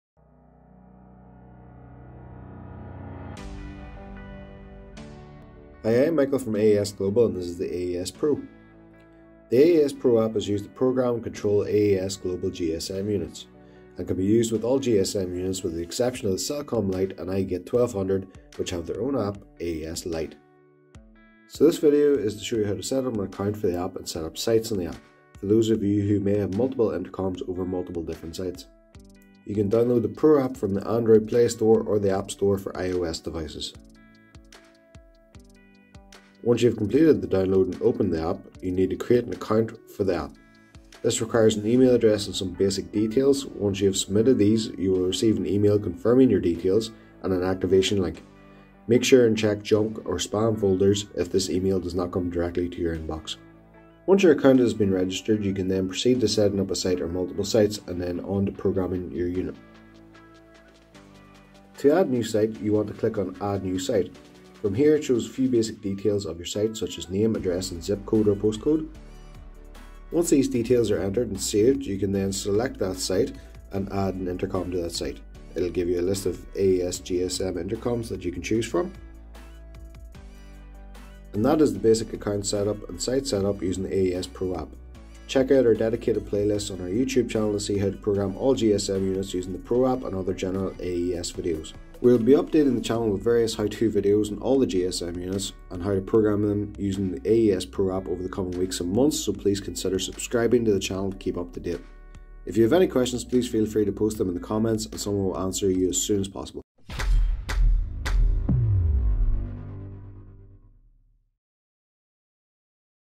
Register and Add Site | AES Pro | Account setup on the NEW Cell/GSM App by AES Global
*EARLY ACCESS AVAILABLE NOW! - ON ANDROID AND APPLE DEVICES*
The first look at our brand new AES PRO app, the new way to program cell/GSM based telephone entry units!
The AES Pro App is designed to make the programming of GSM/Cell-based AES units, easy and hassle-free, taking the often complicated SMS code strings out of the equation by generating them for you.

for everything AES Global.

We Are Social! | Keep up to date with AES Global on your preferred social pages.